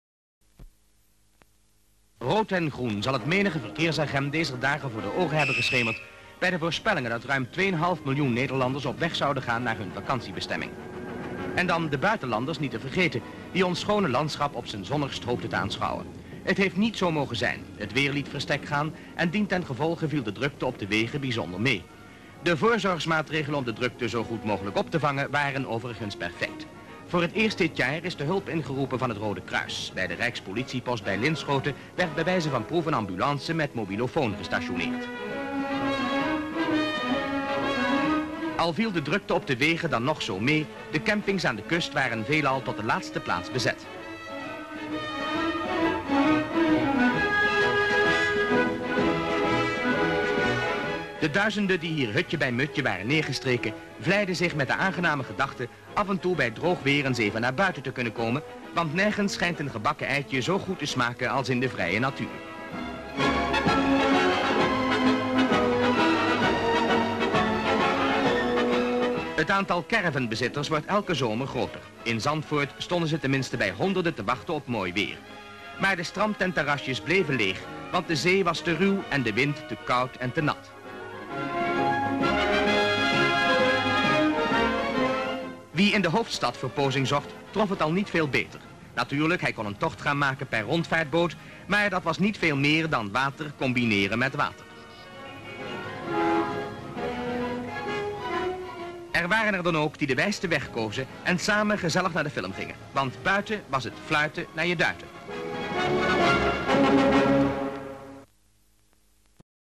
Holiday time (Weeknummer 66-30) 🔴
Description (more): Dutchmen and foreigners go by car on holiday, but the expected crowds remains off by the moderate weather. With a traffic COP that regulates traffic, a mobile red cross point along the motorway at Linschoten, crowded campsites on the coast at Zandvoort and tourists in Amsterdam that a boat trip to and from the film "whistle at you readies".